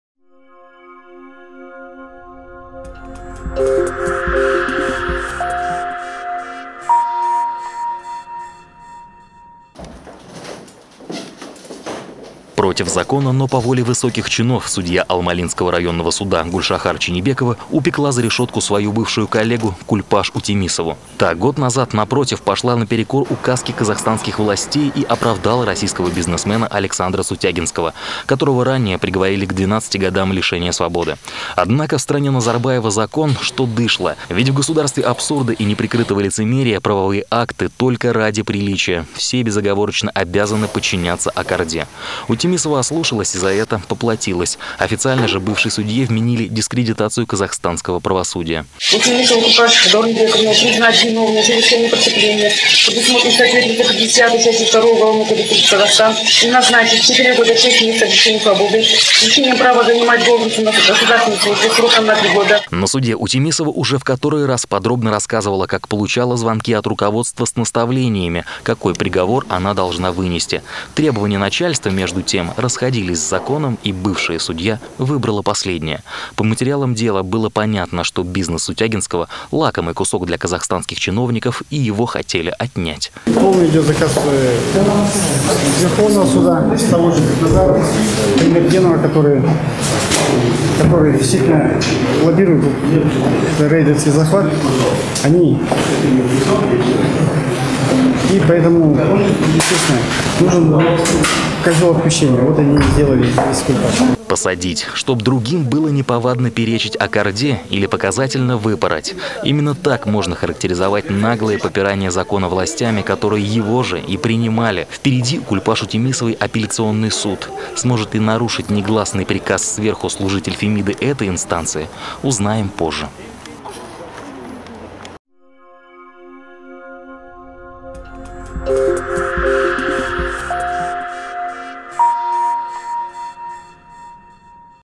Крах казахстанской Фемиды: судью посадили за правду / 1612
Судья Алмалинского районного суда Гульшахар Чинибекова упекла за решетку свою бывшую коллегу Кульпаш Утемисову. Та год назад напротив пошла наперекор указке казахстанских властей и оправдала российского бизнесмена Александра Сутягинского, которого ранее приговорили к 12 годам лишения свободы.